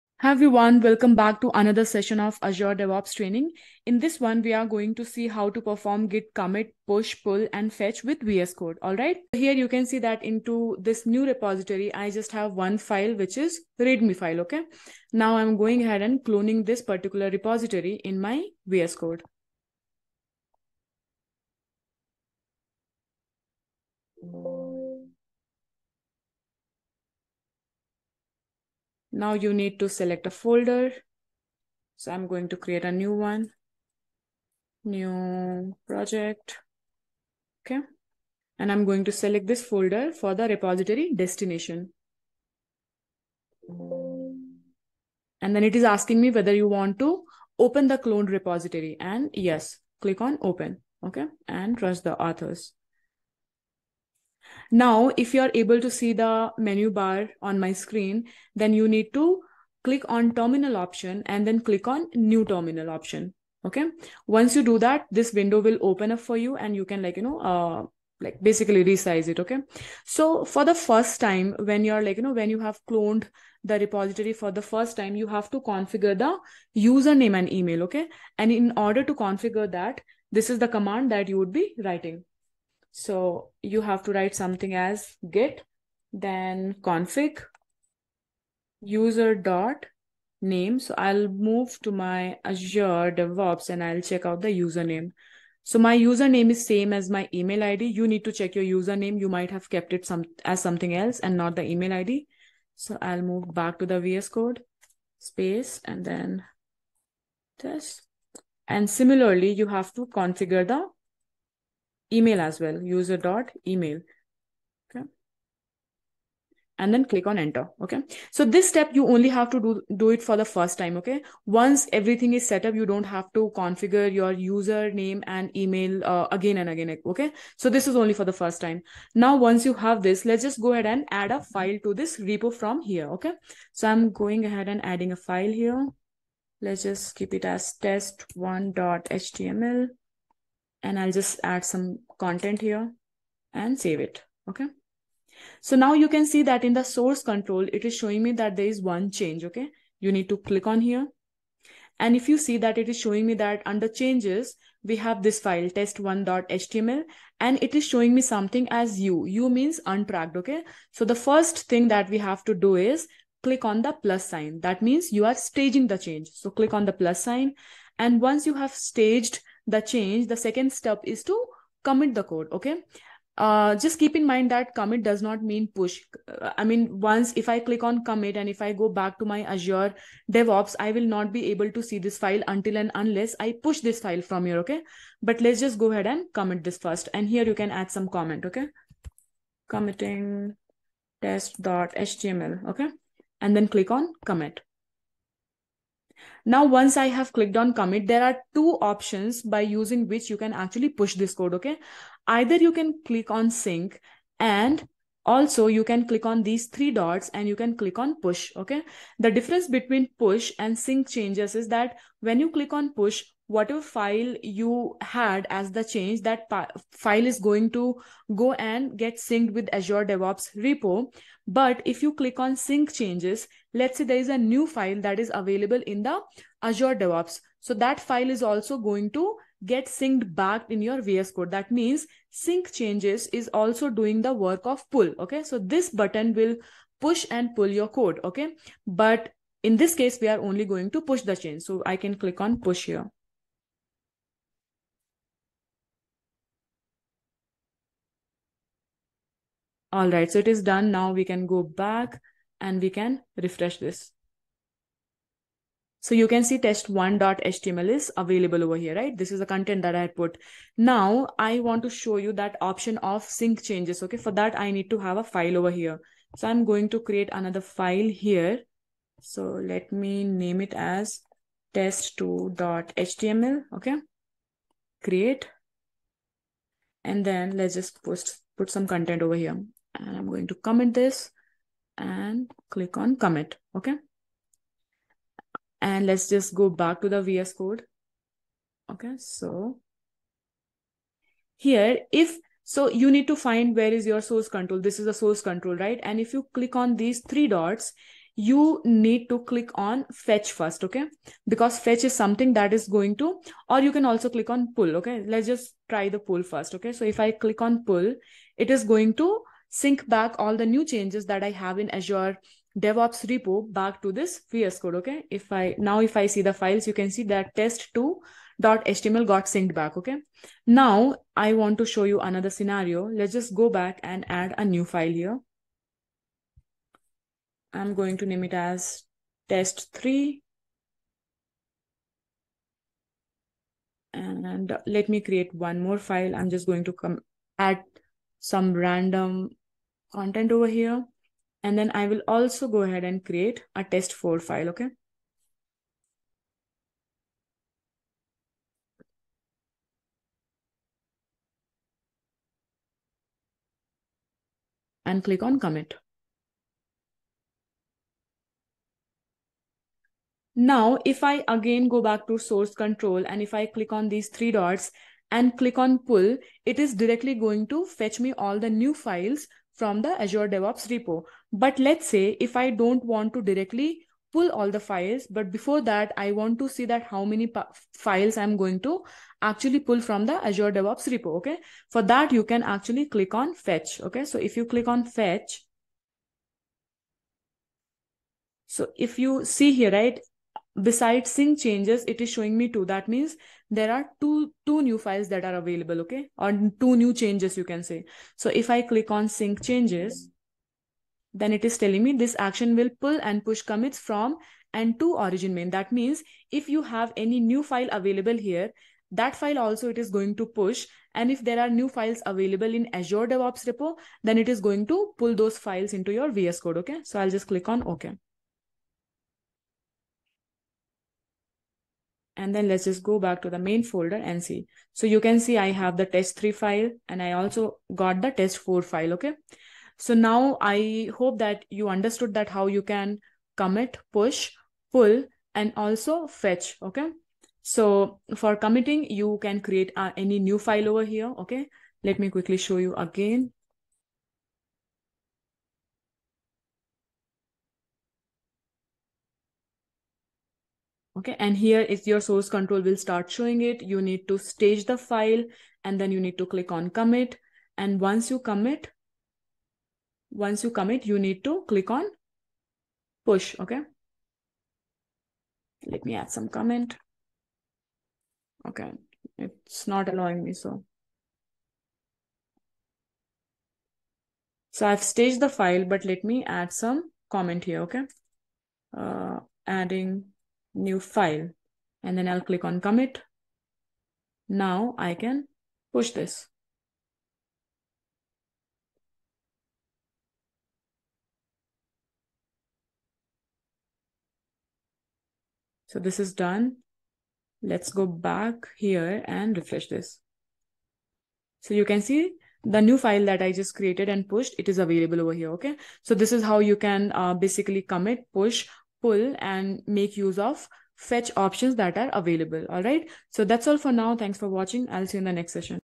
#15 : Git Commit, Push, Pull, and Fetch | VSCode | DevOps | Azure DevOps
Learn how to perform Git commit, push, and pull with VSCode. The video also covers the use of Fetch option.

Topics and Timelines :

0:00:54 : User and Email config set up
0:02:08 : Commit, Push, Pull
0:05:23 : Fetch